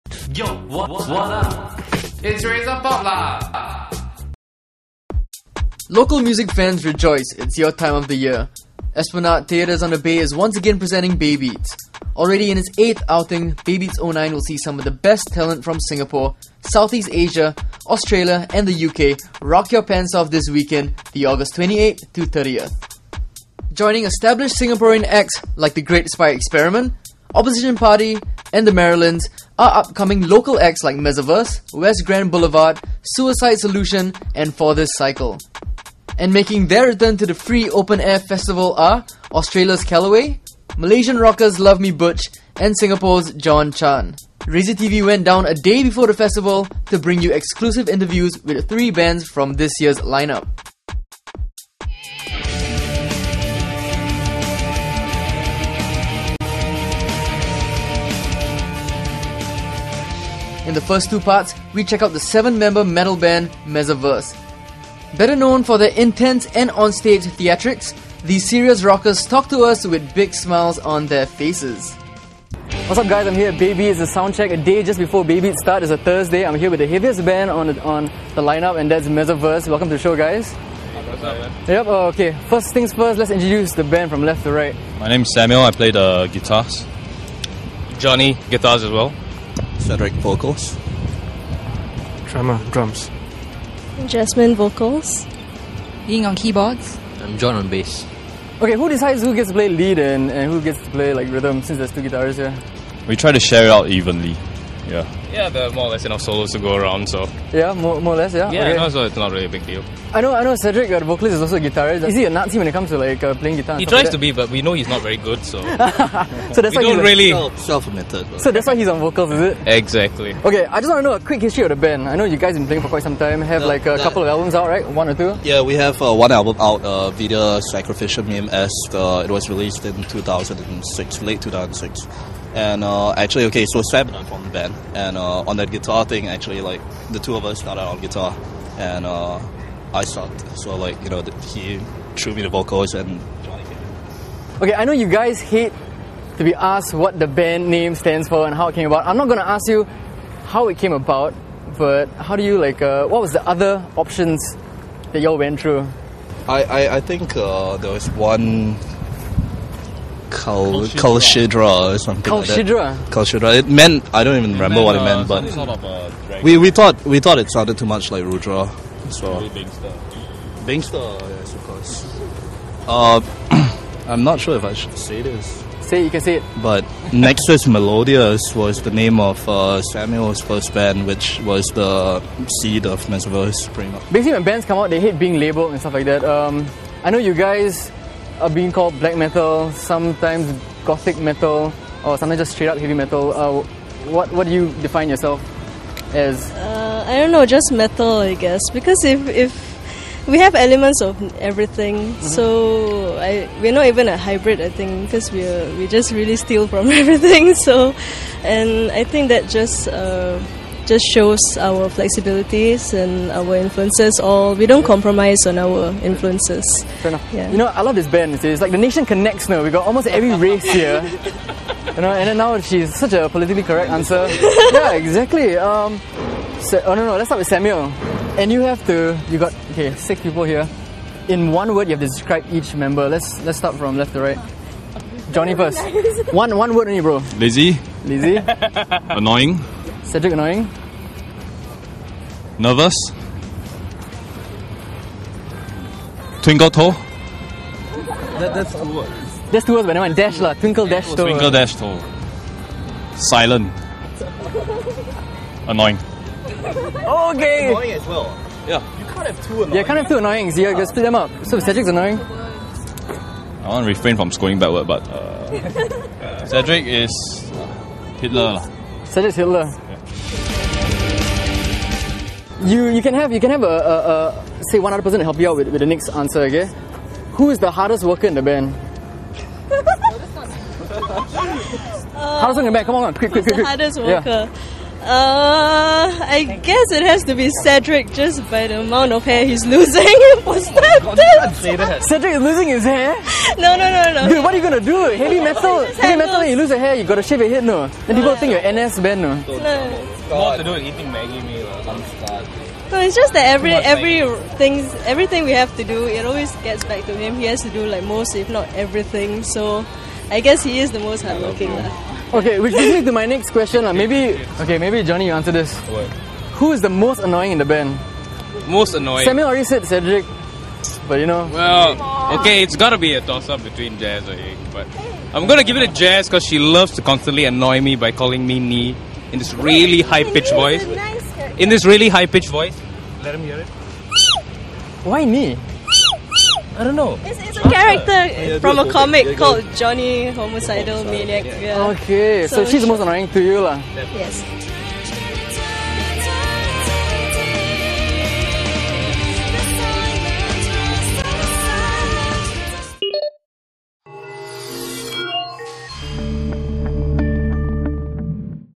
Meza Virs: Baybeat's Heaviest (Baybeats preview Pt 1)
Meet the heaviest band on this year's Baybeats line-up. But before you judge the music, get to know the 7 talented members that make up the heavy metal monster that is Meza Virs. RazorTV unleashes a string of probing questions that might turn the band on each other.